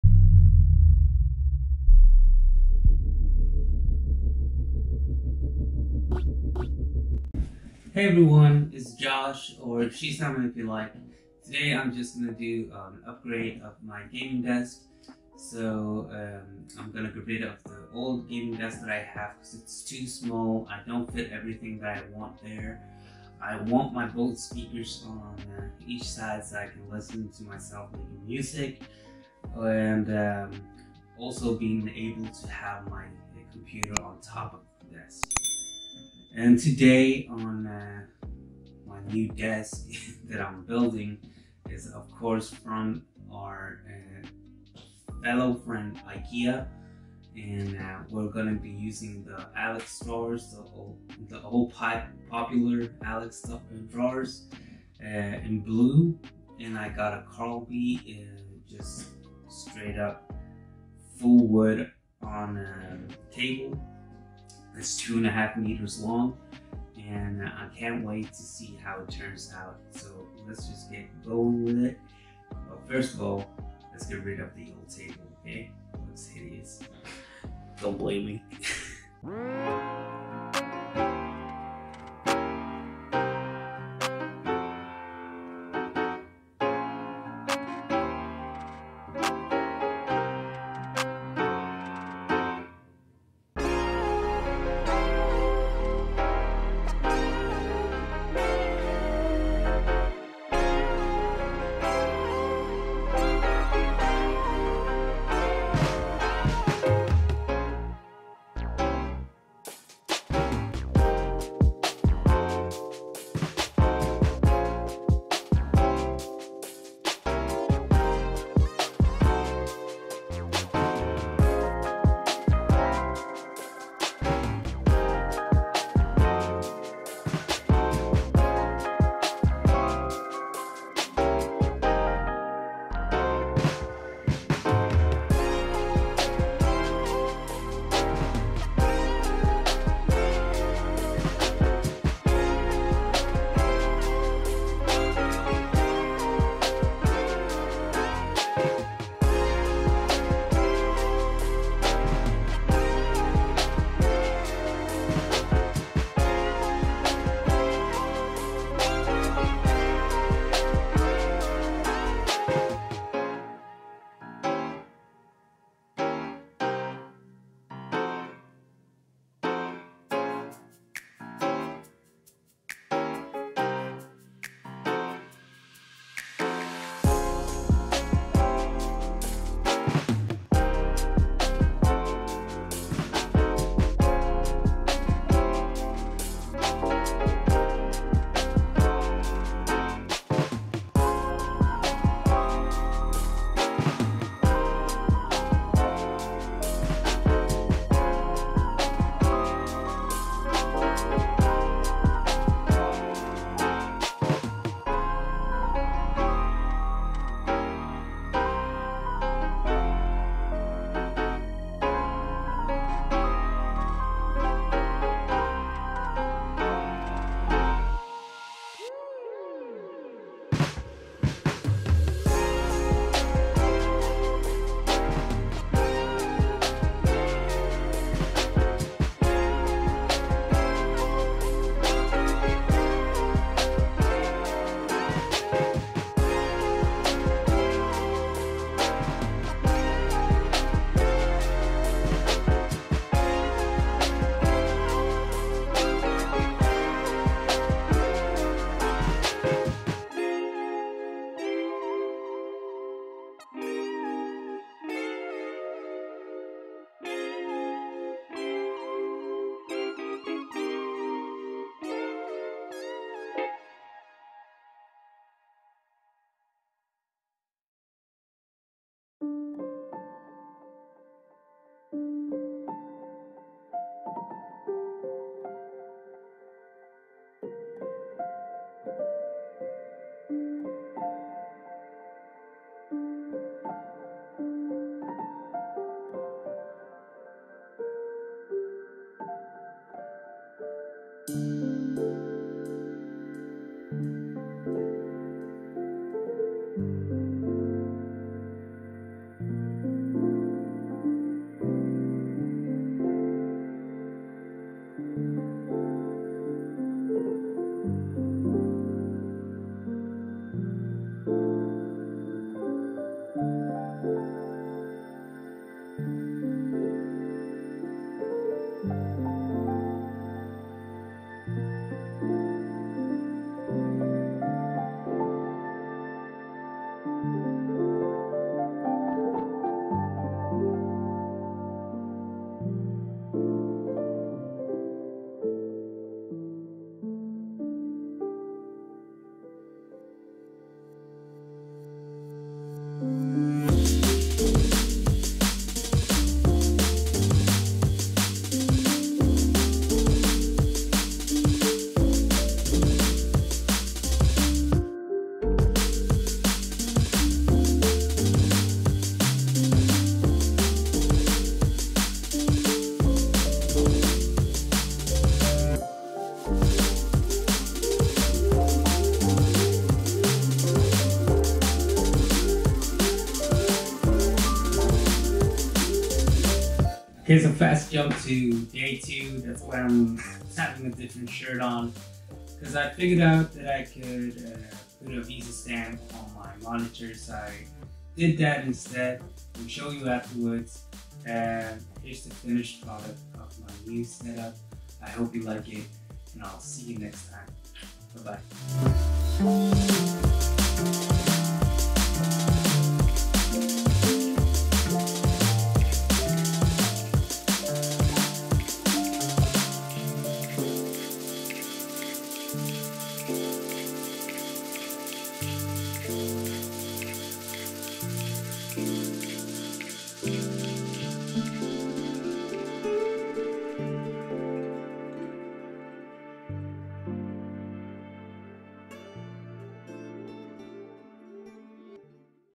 Gaming Setup Upgrade! IKEA Style Gaming Desk. 💯
I finally got my long waited gaming desk upgrade.
I'm super pleased and I hope you like it to!

Welcome to the family: Chef Juice
Help me reach my goal of creating the best community: .. 43% 216/500

I used a KARLBY table and the popular ALEX drawers, and boom it looks so much better. And I have way more space now.
Any question please leave it in the comments down below, peace ^_^